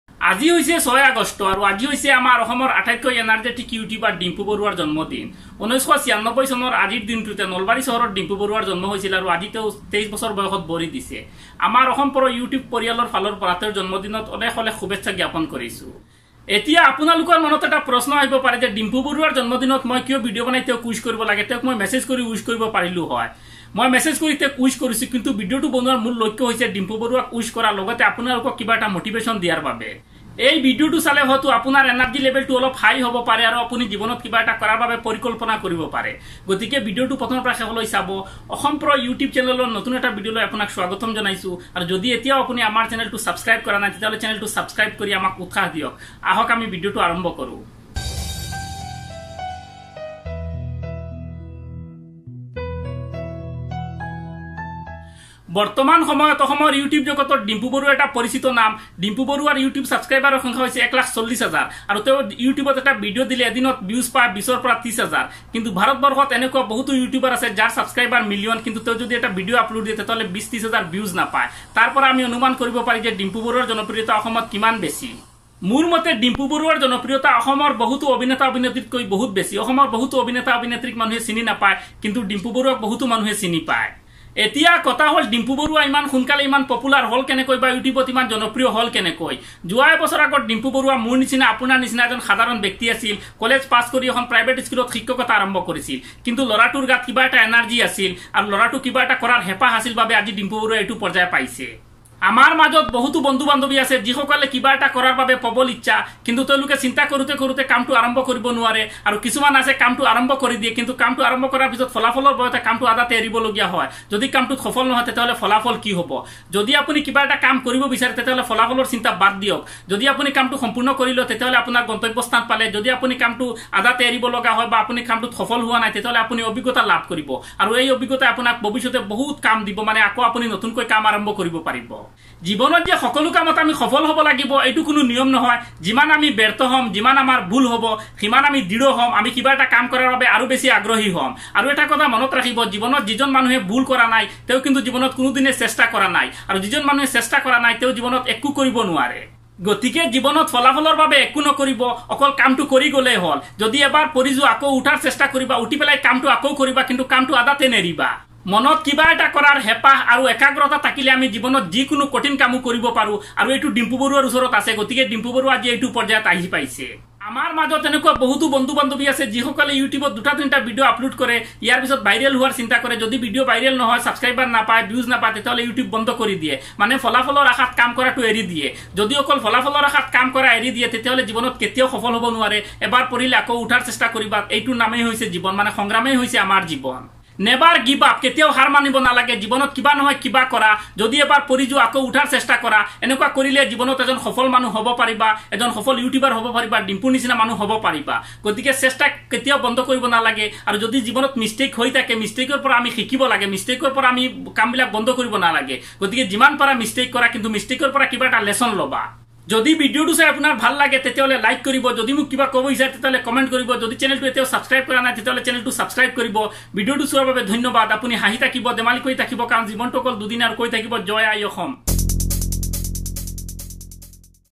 How to be a successful person. Assamese Motivational video ft. Dimpu Baruah. Assam pro
Today I'm going to talk how to become a successfull person. So watch this video.

You can follow us onq social media.